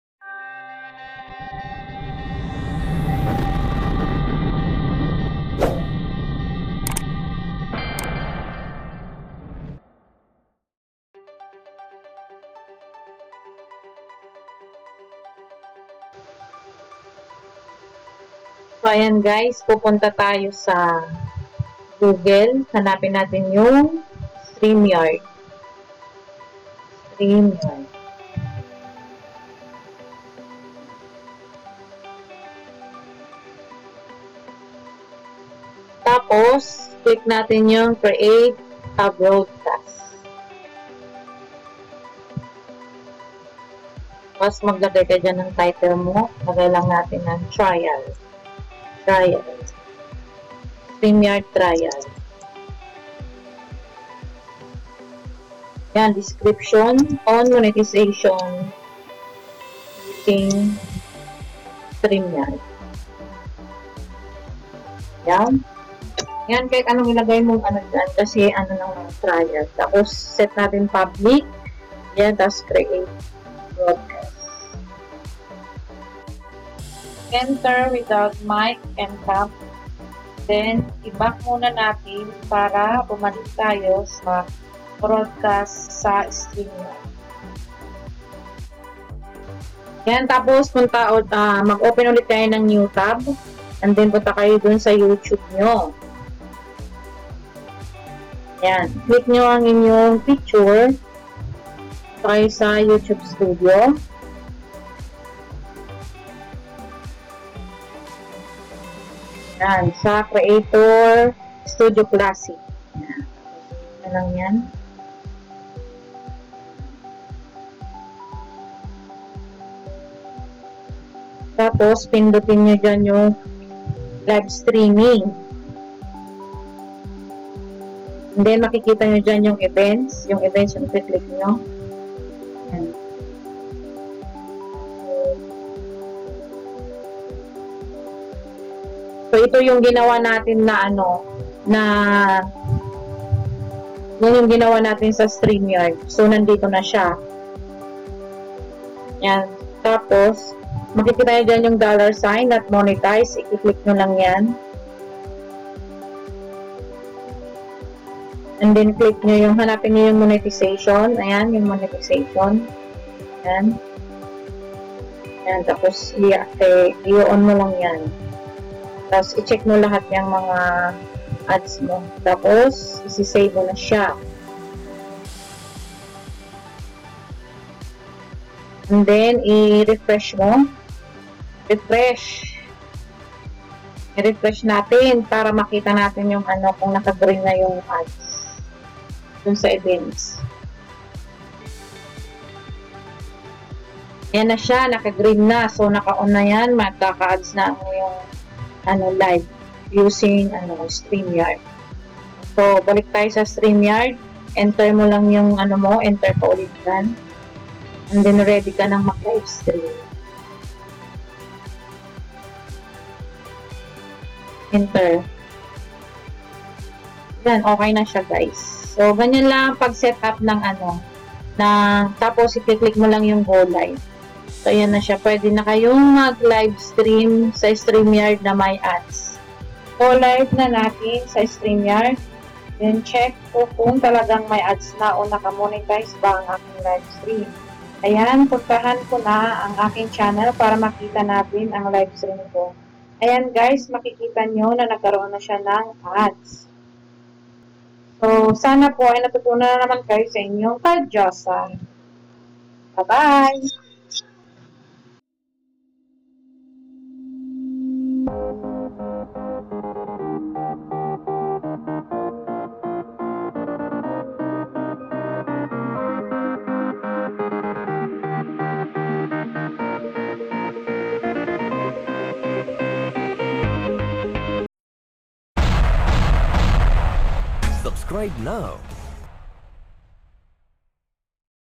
HOW TO ENABLE YOUR ADS WHEN USING STREAMYARD | See Description for 2020 Update
HOW TO ENABLE YOUR ADS WHEN USING STREAMYARD

How to Enable Ads Using Streamyard?

Update 2020 (New Youtube Studio)
1. Login your account to Streamyard.
2. Create a broadcast
3. Go to your youtube Channel
4. Go to Youtube Studio
5. Go to "Go Live"
6. then go to "Manage"
7. "ON" your Monetization with self rate (for Safe Ads, to avoid yellow dollar sign)
8. then Go Live in your Streamyard